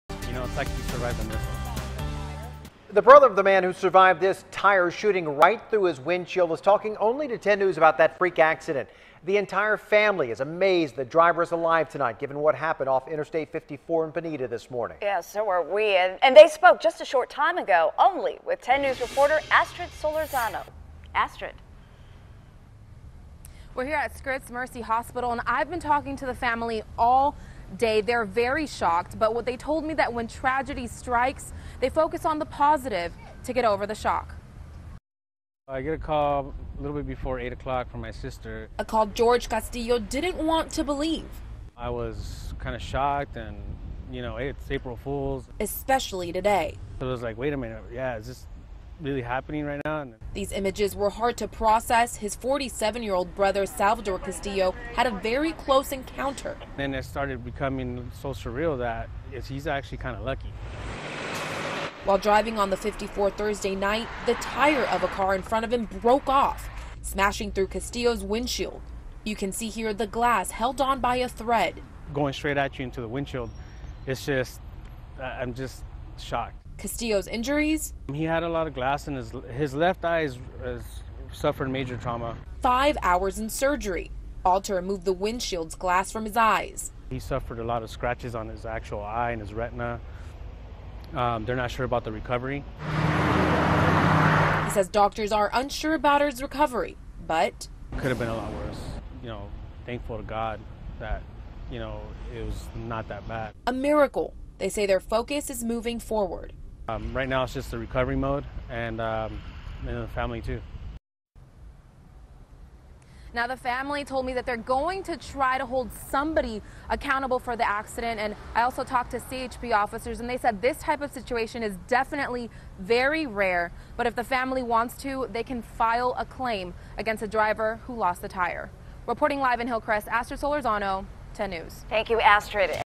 Tire flies through windshield 'like a missile'
Family of the victim is amazed he's alive tonight.